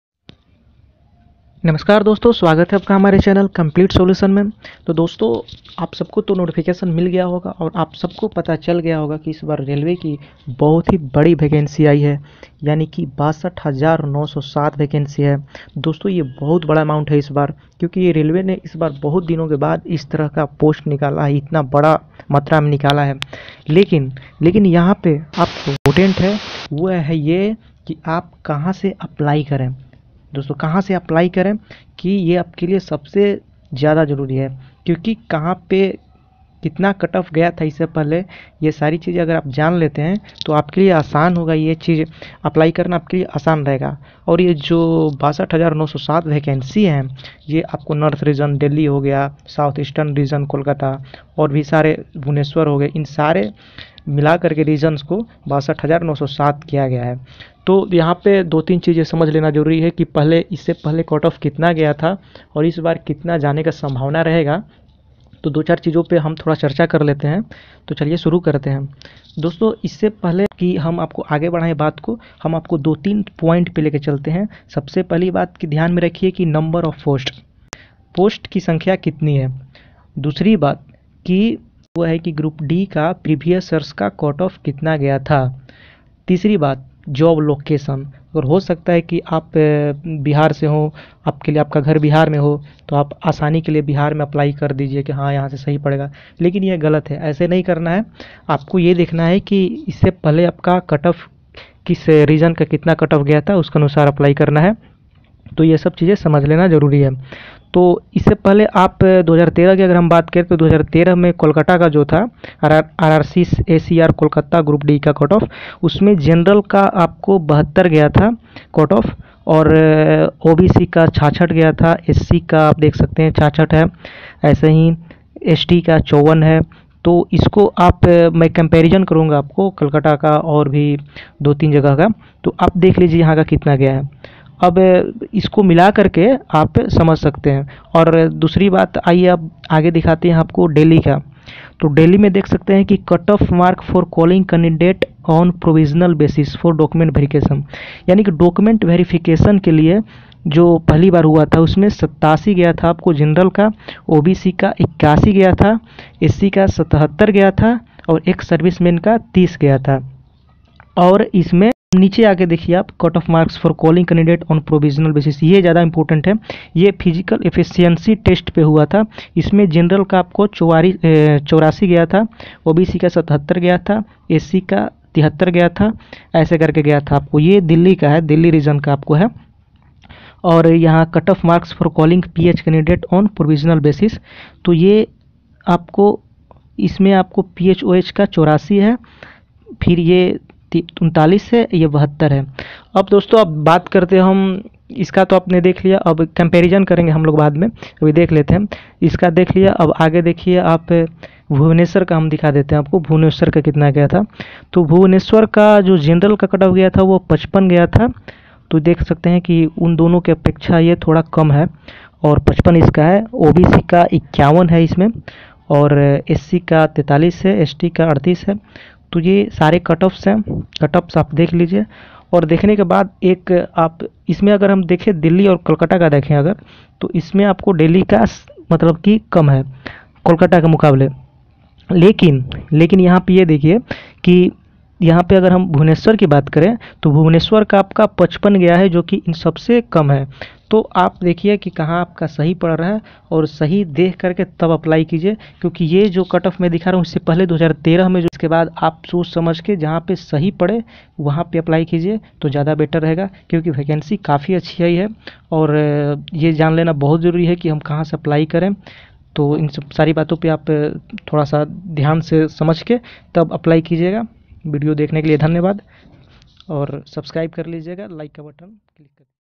किस बोर्ड से भरें railway group D exam previous year cutt off rrb vacancy
in this video i am going to talk about railway relate some notification and about railway syllabus, group D salary, and railway group-D cutt off cut off and also about railway group D vacancy post, and pay scale